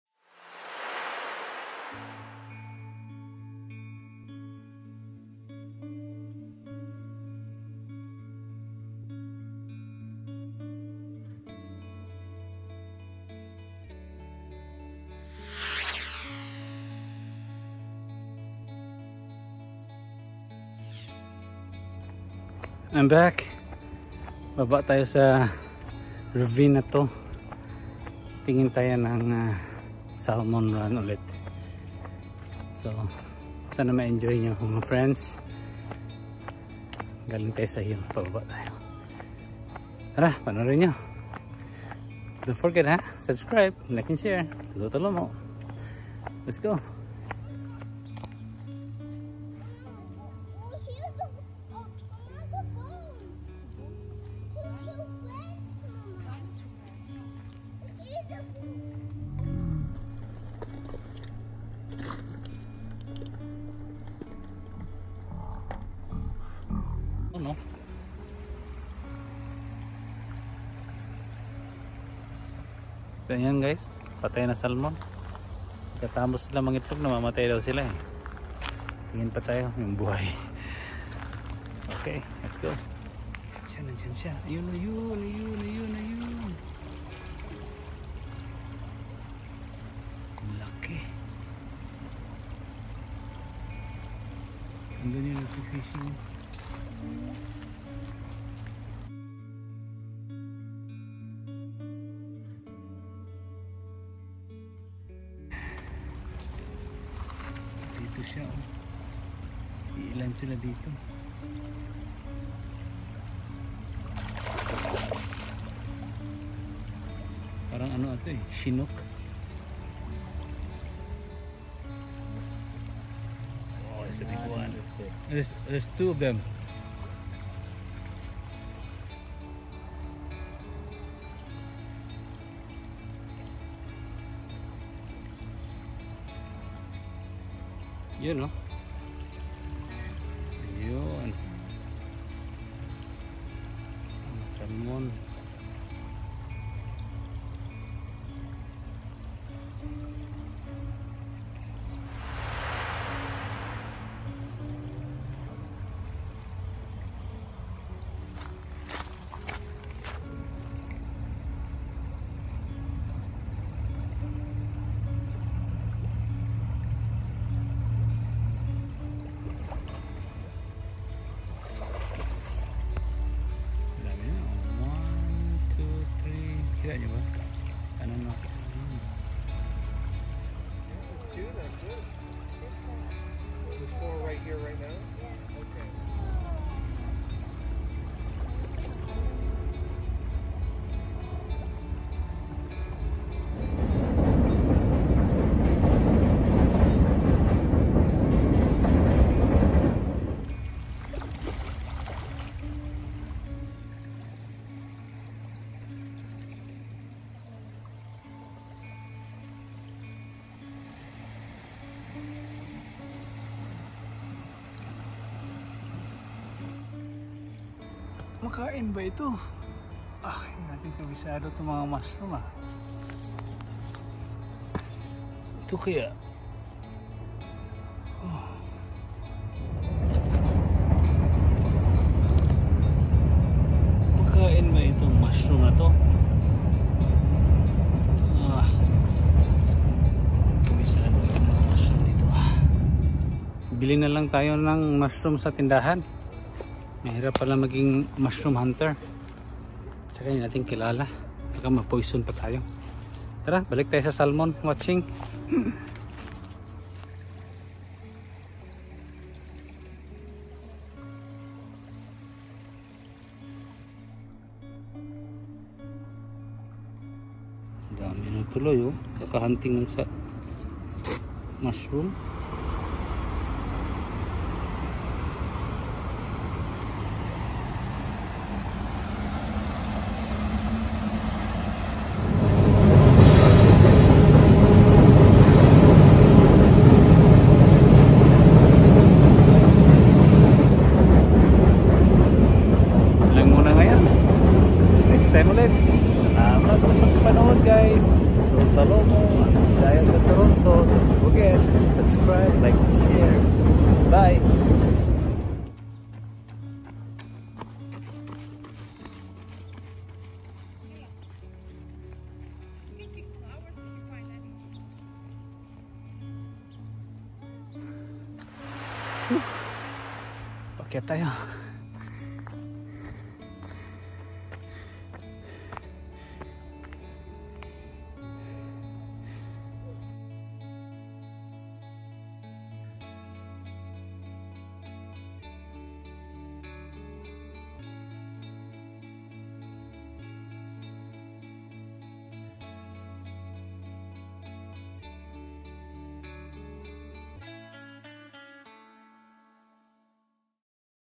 Watching Chinook Salmon run
It’s the season of Salmon run, I just want you all to see this. It’s amazing and also,I was checking some wild mushrooms in the forest but I did not collect them just looking. Please watch it in full and enjoy.